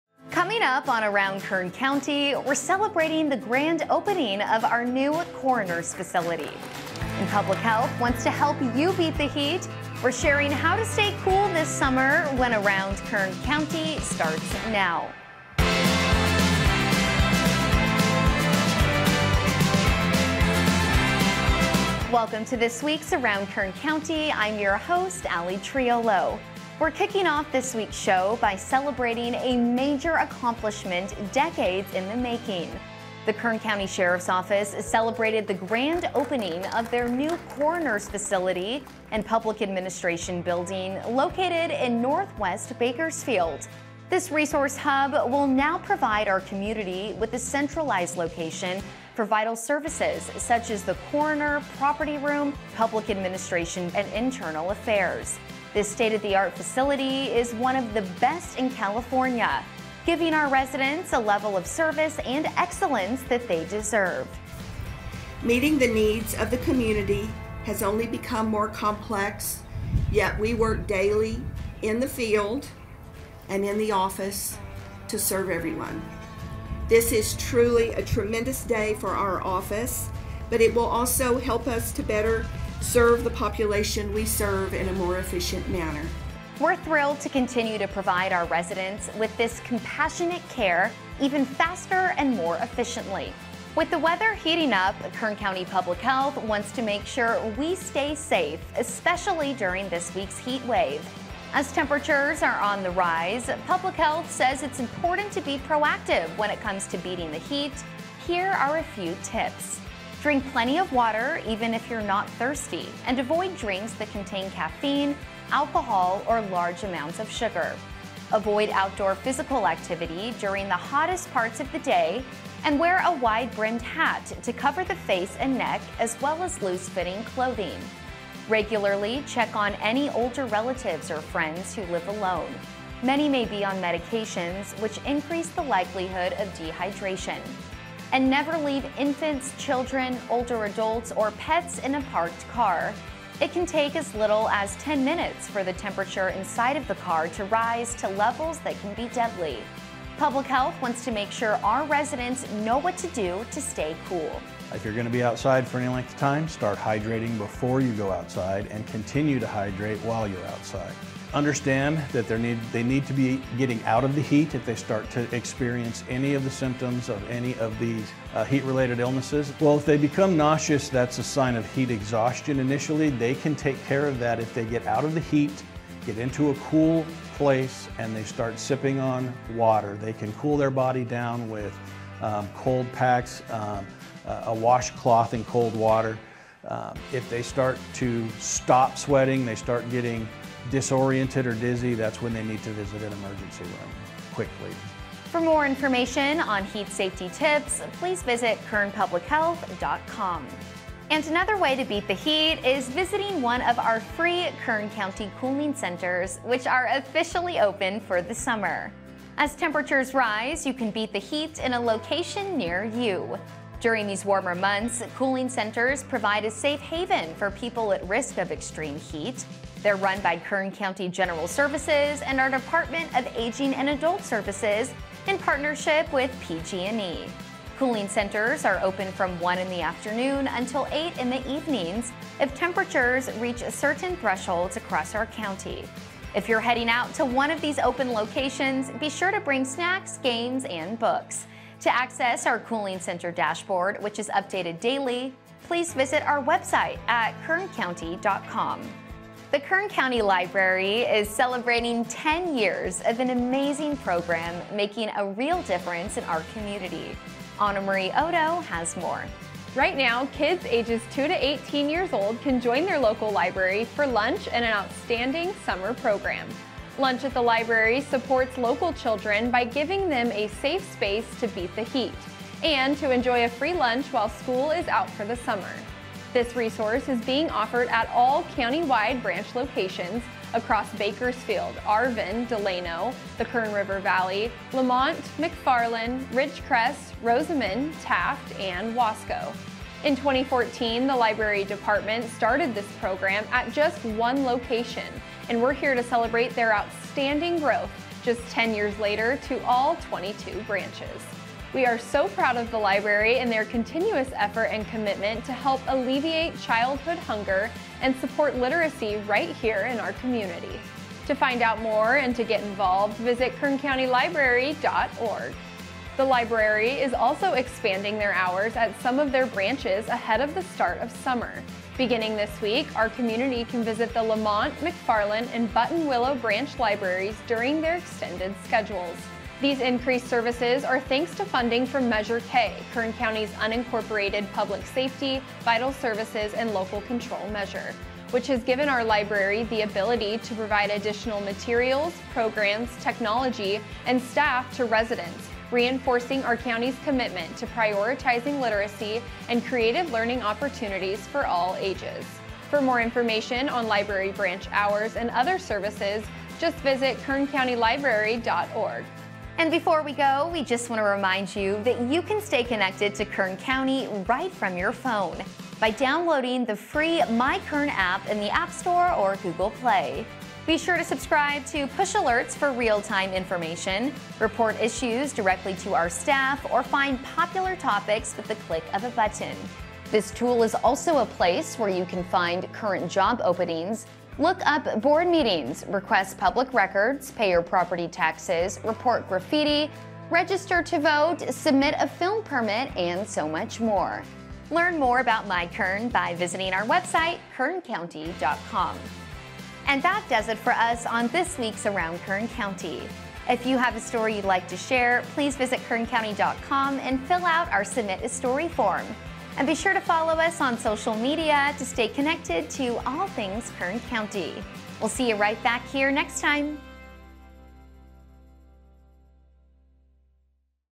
Around Kern County, Episode 92
On this week's Around Kern County, we're celebrating the grand opening of our new coroner's facility, highlighting important heat safety tips and resources, and showcasing summer programs happening across our libraries.